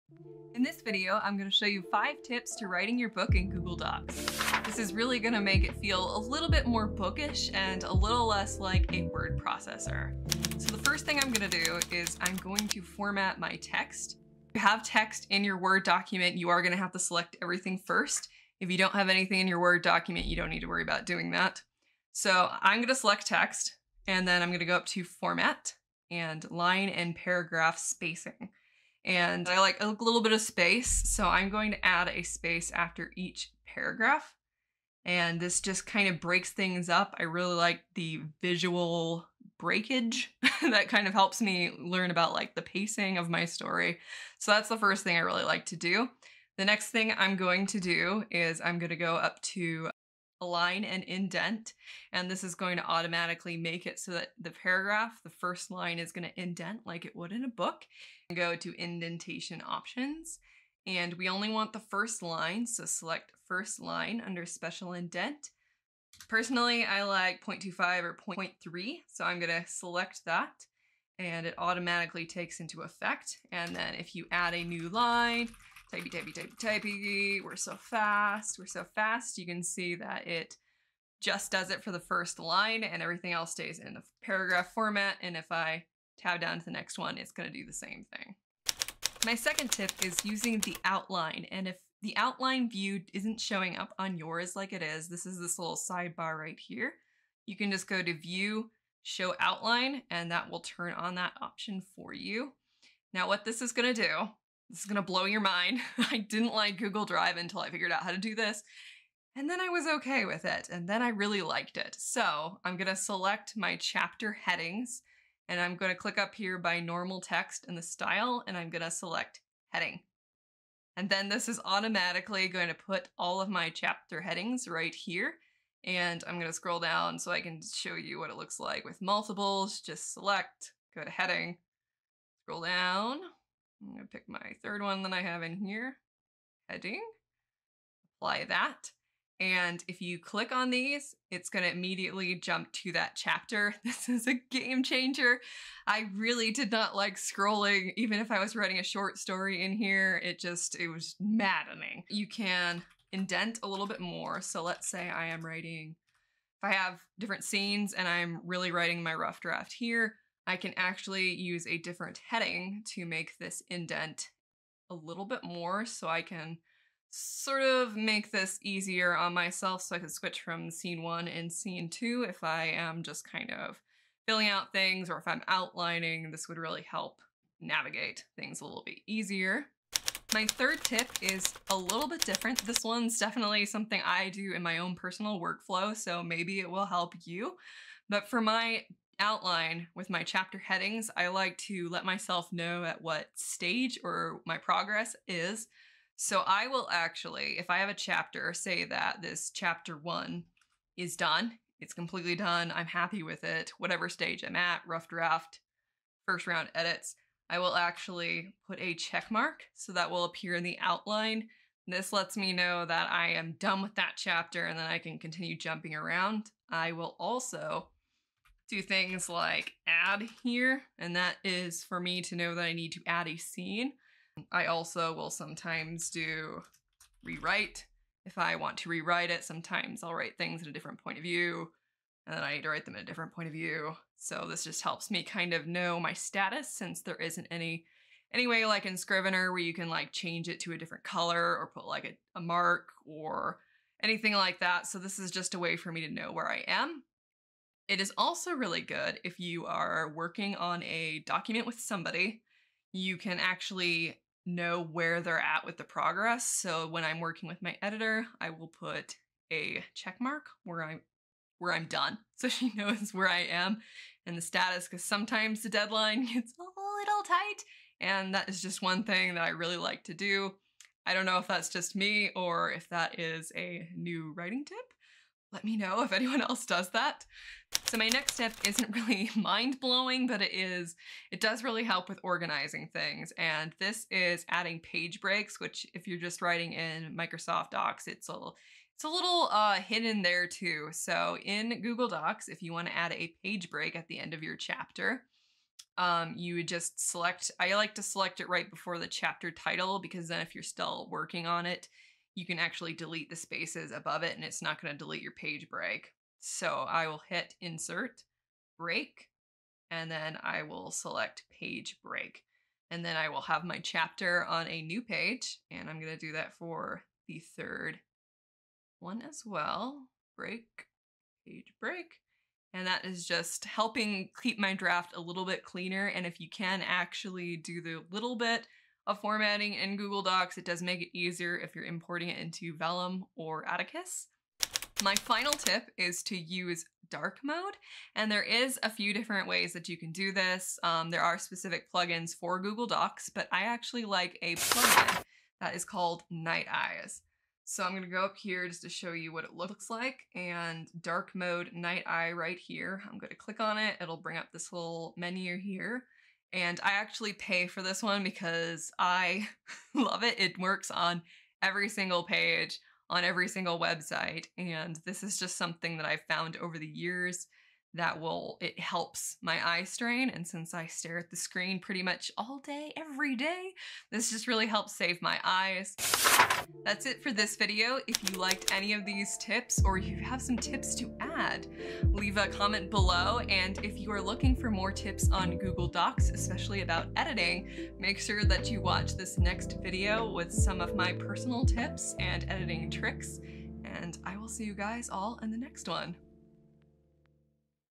5 Tips for Writing a Book in Google Docs
If you're new to Google Docs or need some tips for writing a book, this video is for you. I'll share tips on how to make your manuscript look more bookish, and how to utilize Docs to its fullest potential, from creating chapters to formatting your book correctly. By the end of this video, you'll be writing book like a pro!

Chapters:
00:00 - Intro
00:10 - Formatting the body text
01:28 - Using the outline to navigate
02:59 -  Add notes to your outline
04:37 - Page breaks
05:34 - Dark mode
06:16 - Outro